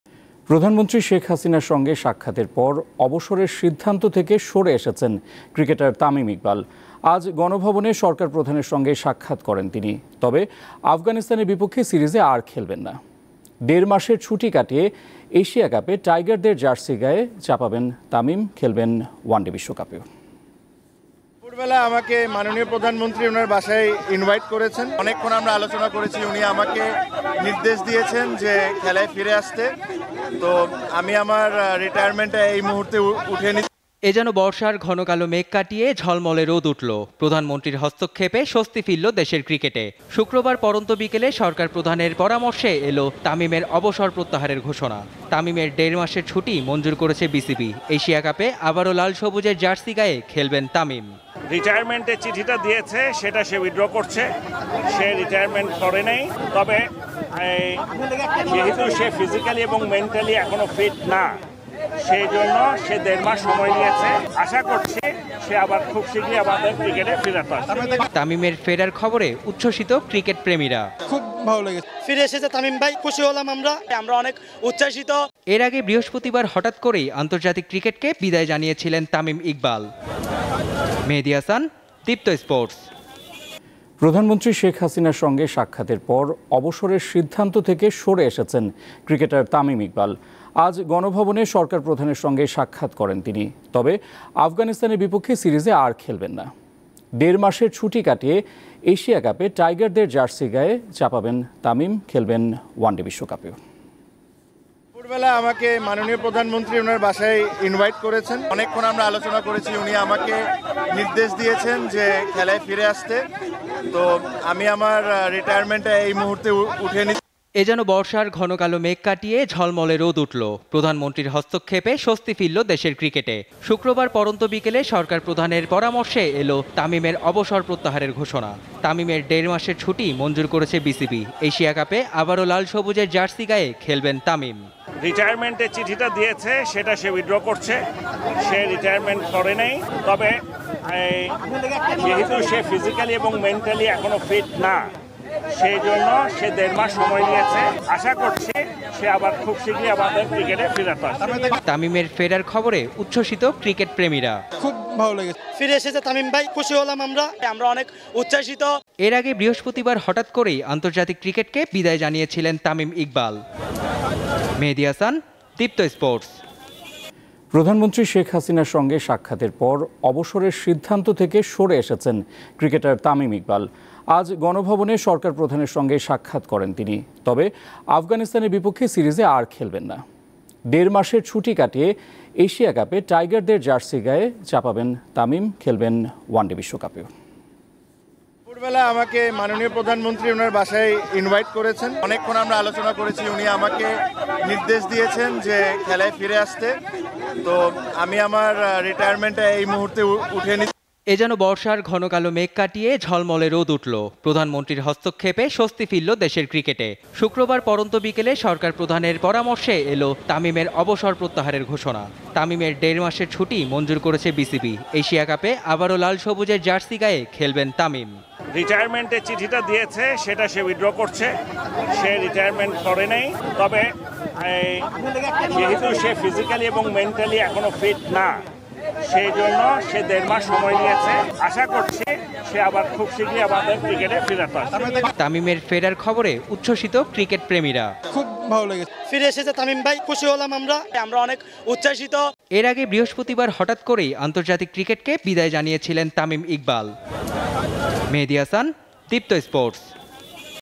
প্রধানমন্ত্রীর অনুপ্রেরণায় মাঠে ফিরলেন তামিম | Sheikh Hasina | Tamim Iqbal | Deepto News
প্রধানমন্ত্রীর অনুপ্রেরণায় মাঠে ফিরলেন তামিম

Address:
Deepto TV
7/A/GA Tejgaon Industrial Area,